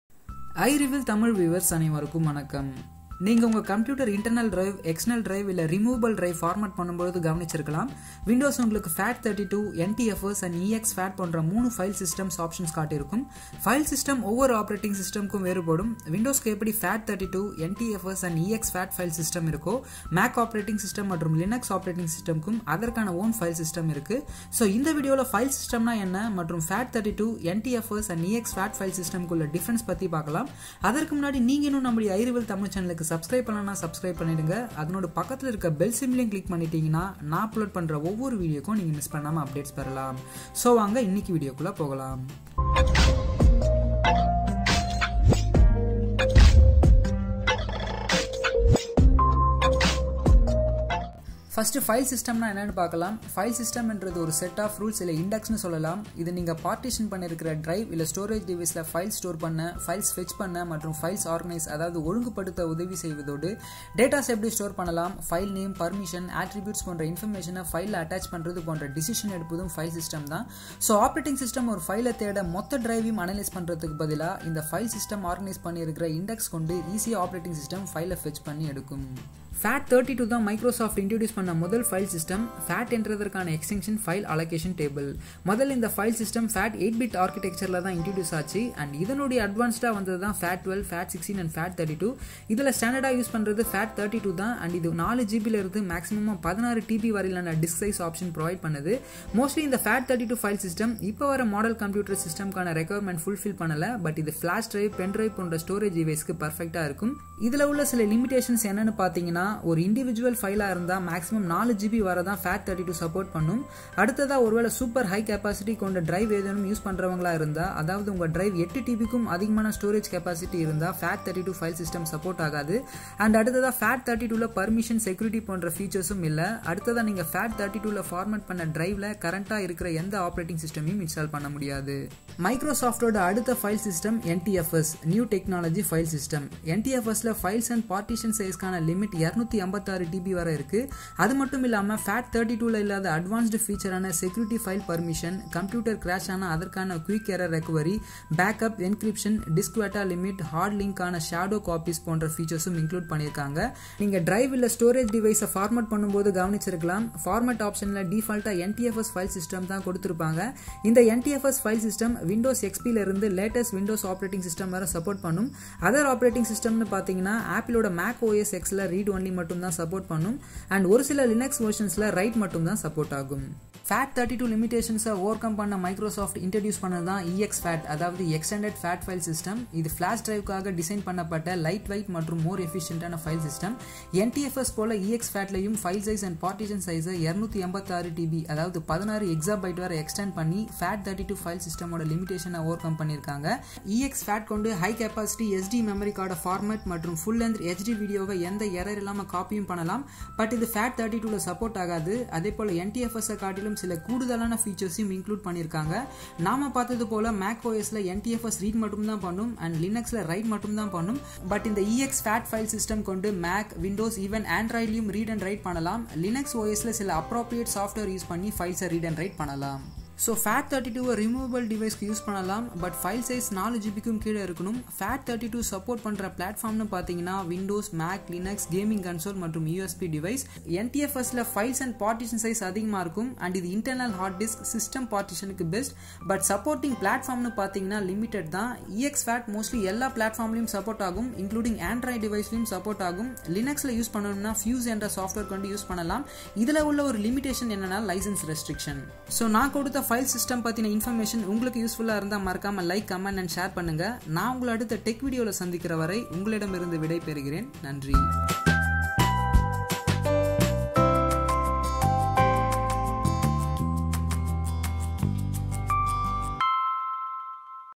FAT32 VS NTFS VS ExFAT | Windows File System Explained in TAMIL
File System Concept is one of the Basic you should Know When you Do Partitions on your Drive or Formatting your Internal, External and Flash Drives.
In this Video I've Explained you What is File System and the Differences in FAT32, NTFS and ExFAT File System